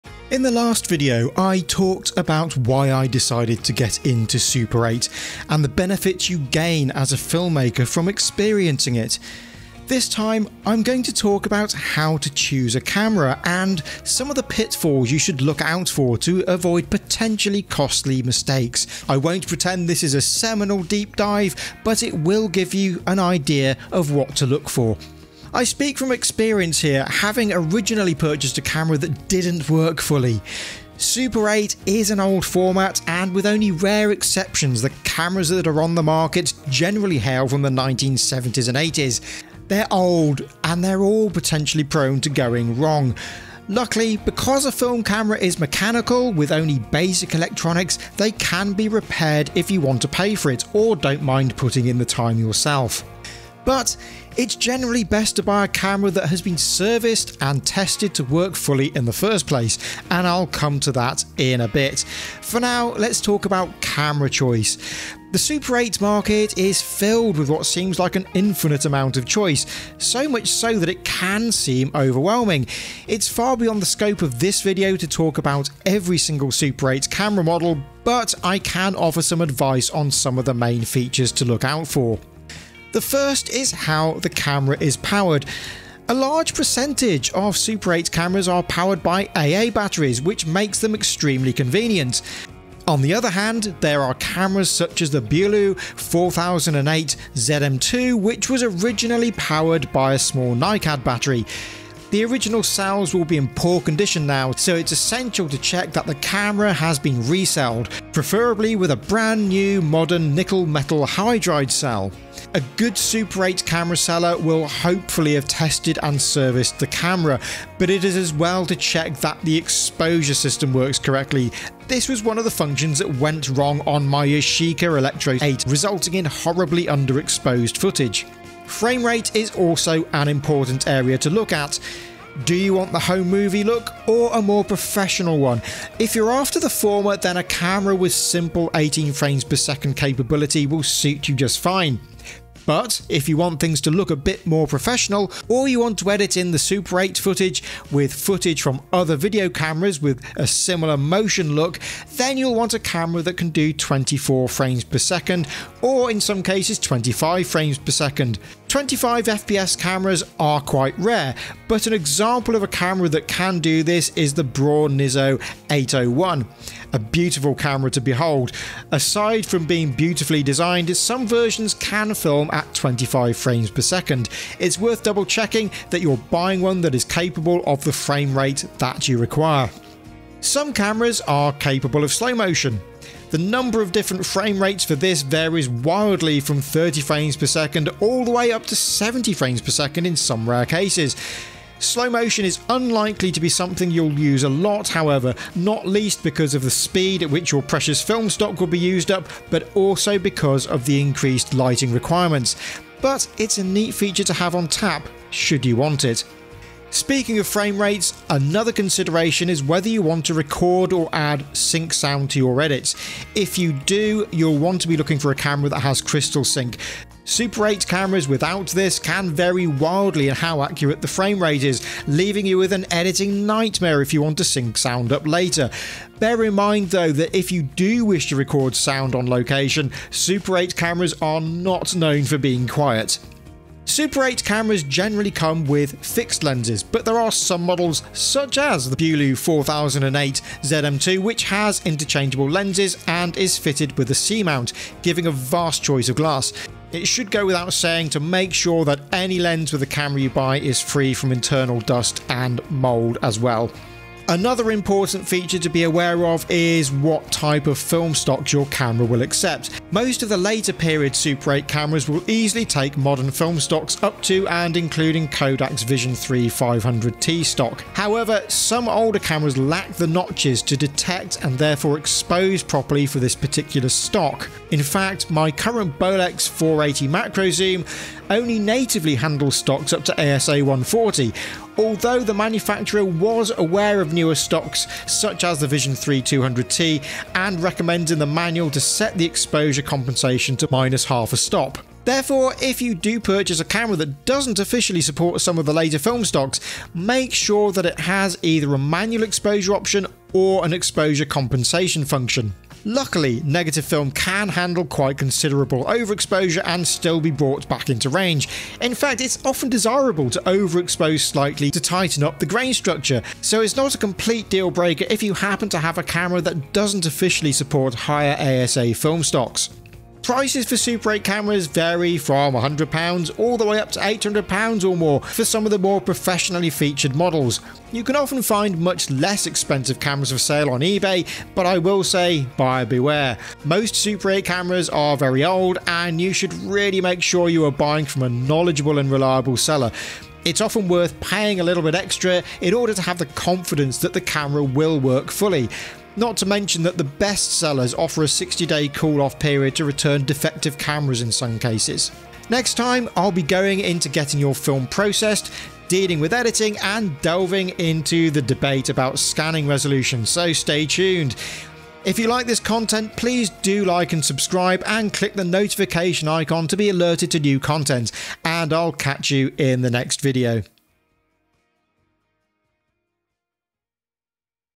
Starting with Super 8 - How to Choose a Camera
In the second part of our series on Super 8mm, Simon takes you through the process of choosing a camera, and what to look for, as well as the pitfalls to avoid.

00:00: Introduction
01:23: Batteries
02:00: Exposure system
02:08: Frame rate choice
02:59: Slow motion
03:26: Sync sound
03:53: Lens choice
04:14: Film stock compatibility
05:20: Camera prices